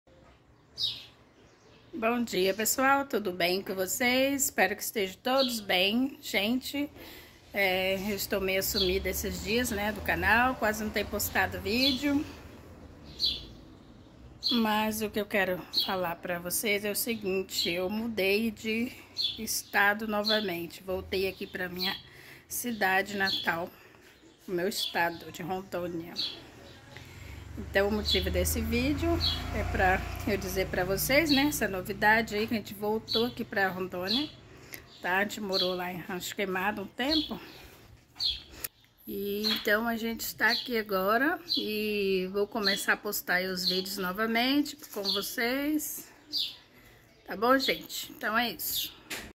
SURPRESA!!! MUDEI DE ESTADO! @Canal Aprendiz
Olá pessoal resolvemos mudar de volta pra nosso estado.
a decisão foi muito rápida e nem tive tempo pra fazer vídeos falando sobre isso, porque tive pouco tempo pra ajeitar as coisas da mudança.